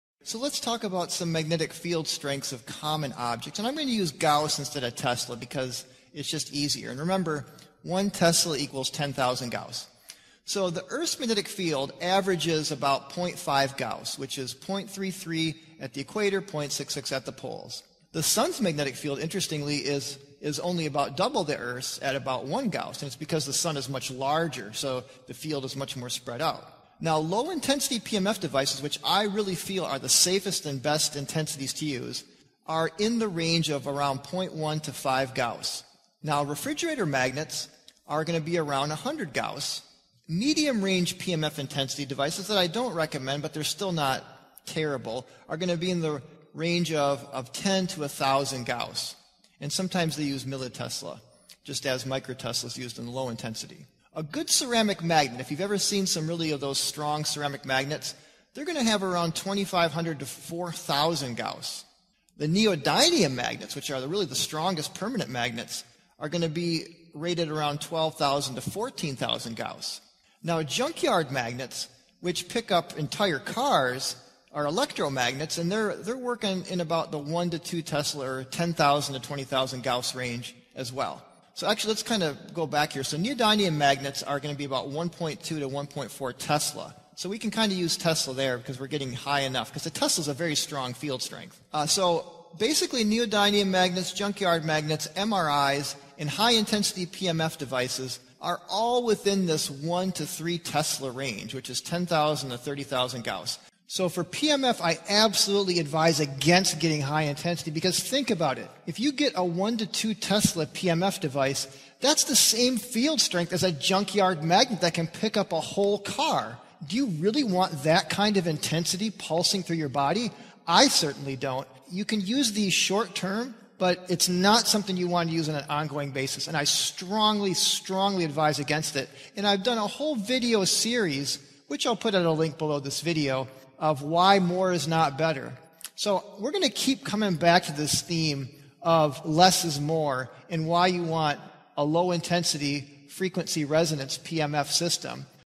Understanding PEMF / Magnetic Field Intensities & Why LESS is More!
Useful Conversions
1 Tesla = 10,000 Gauss
1 uT (microtesla) = .01 Gauss
100 uT = 1 Gauss
Earth = .30-.70 Gauss = 30-70 uT = Great Choice for PEMF!
The Earth and Safe and Effective PEMF devices are ideally the range of
1-500 uT = .1-5 Gauss. These levels are ALSO considered safe by ICNIRP standards
for LOW frequencies (which is what you want).

The key is frequency resonance - NOT INTENSITY!!

Magnetic Field Intensity is a common source of confusion with many people I talk to about PEMF devices.

The main units used are Gauss and Tesla. Gauss is the gaussian unit of cm/sec/grams where Tesla is the SI unit of meters/sec/kilograms.

This short excerpt shows some examples of common magnetic field sources and their intensities to give you an intuitive feel for Gauss and Tesla.

To Understand the Real Physics of PEMF Therapy, watch my playlist on the Basic Science of PEMF.